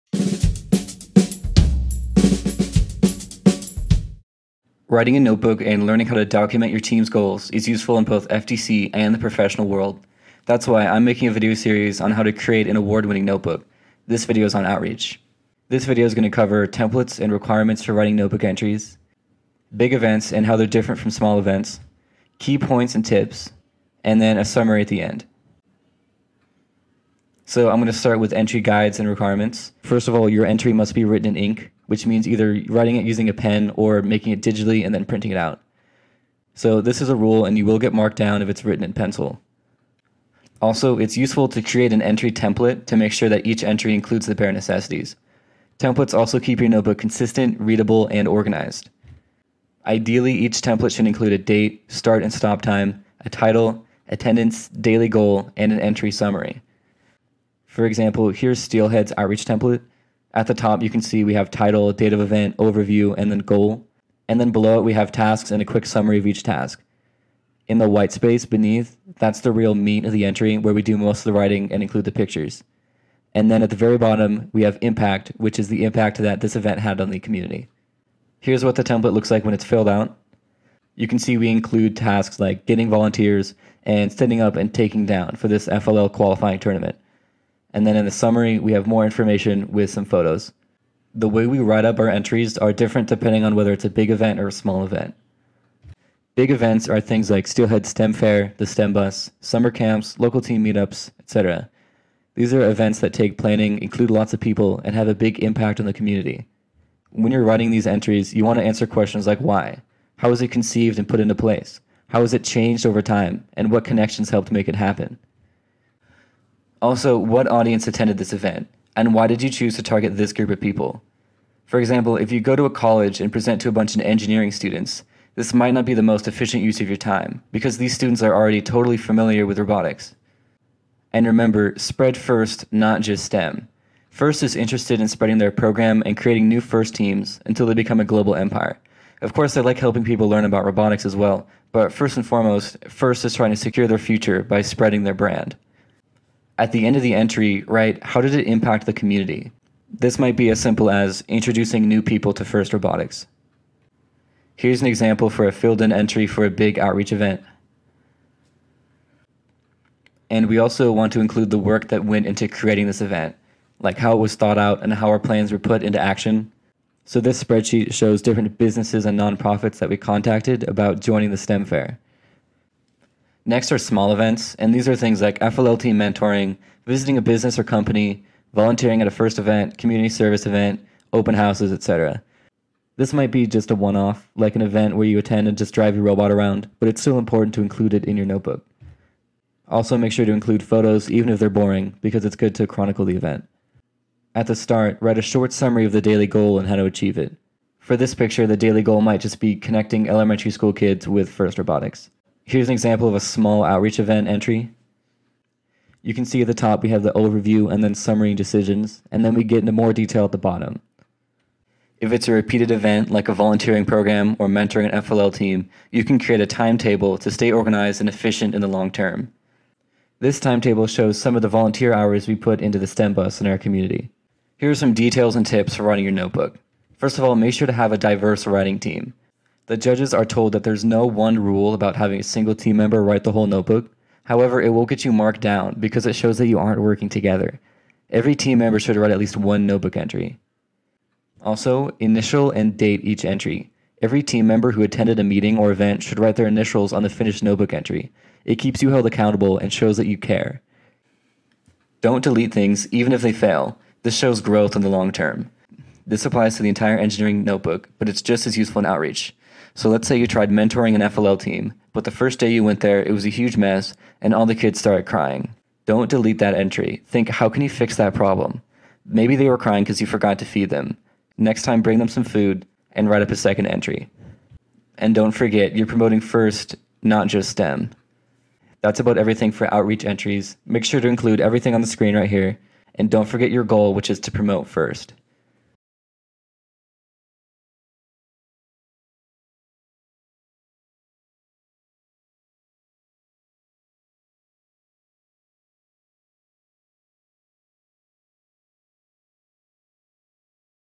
FTC Engineering Notebook Tutorial: OUTREACH
FTC First Tech Challenge engineering notebook example tutorial for writing an Inspire 1 award-winning outreach notebook entry. This engineering notebook guide should help your team write a great notebook for your next competition! This video is centered on OUTREACH.

The FIRST Tech Challenge is designed for students in grades 7–12 to compete head to head, by designing, building, and programming a robot to compete in an alliance format against other teams.